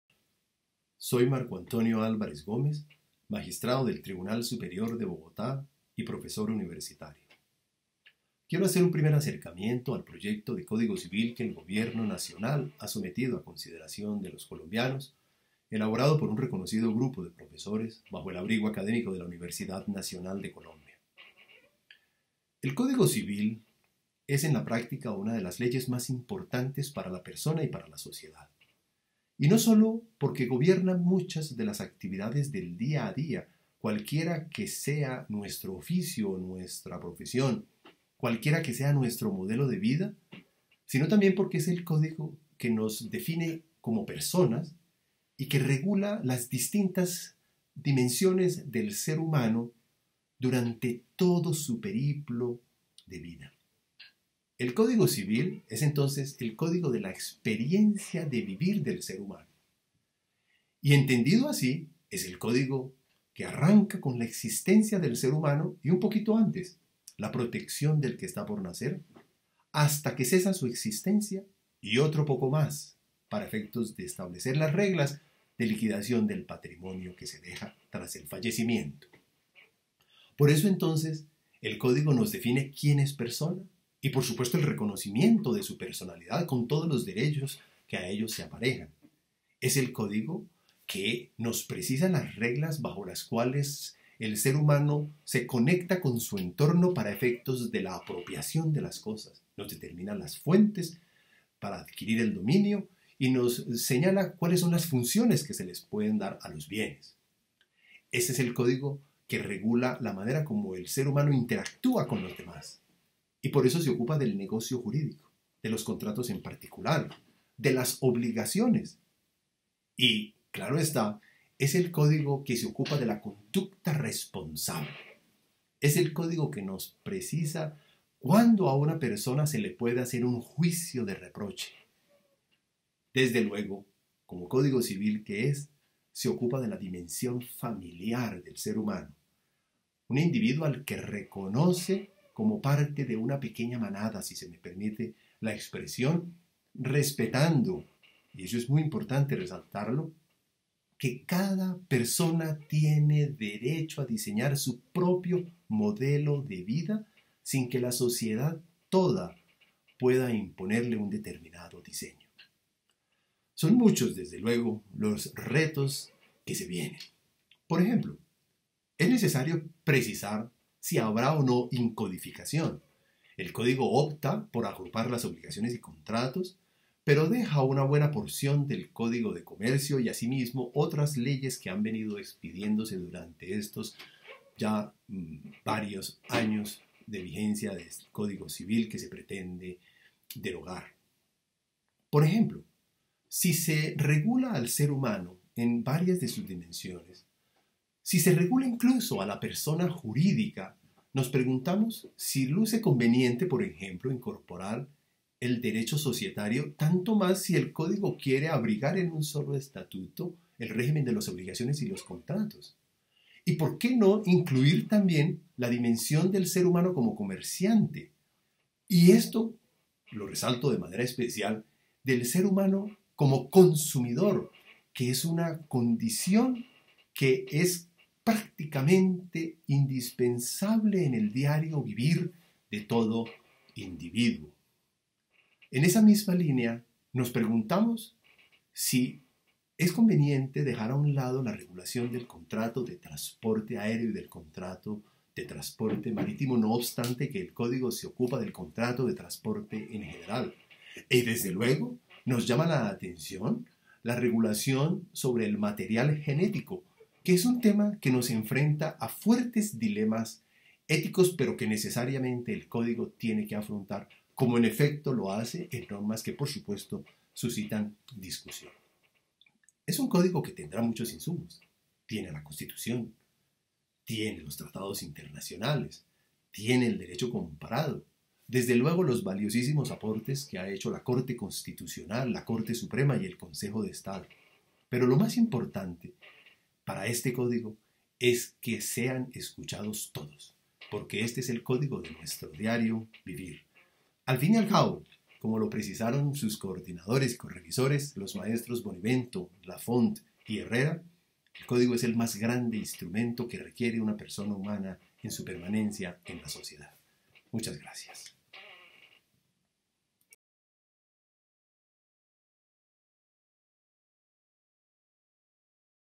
Videocolumnista: Marco Antonio Álvarez
VIDEOCOLUMNAS Invitamos a distintos expertos en Derecho Civil para que analizaran y opinaran respecto a la iniciativa legislativa del nuevo Código Civil de Colombia.